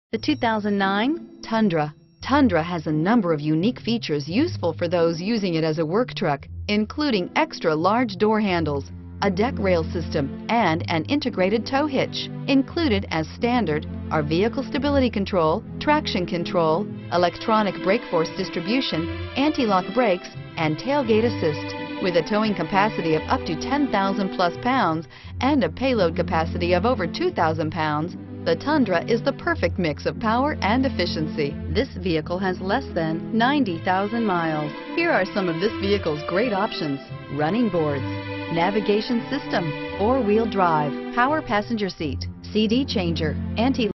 2009 Toyota Tundra 4WD Truck Live Video! Tampa, Wesley Chapel, Brandon, New Port Richey, FL Live 16
2009 Toyota Tundra 4WD Truck LTD

Stadium Toyota
5088 N. Dale Mabry Highway

Meet our 2009 Toyota Tundra Limited CrewMax  4X4 offering rugged underpinnings, loads of brawn, Toyota reliability, impressive safety features, and acres of interior space all add up to one heck of a truck. Under the hood is the sought after 5.7 Liter i-Force V8 with dual independent variable valve timing connected to a purpose built automatic transmission. The power-train is smooth, steady, alert, and reliable! When tasked with the heavy-duty work this 4x4 does so well because its beefy chassis will tame those beastly bumps out there.     The Cabin in our Limited CrewMax is nice and roomy. Controls are large enough and well-marked. Check out the leather seats, Navigation, Sunroof, and more!    It's big, it's powerful, it's yours!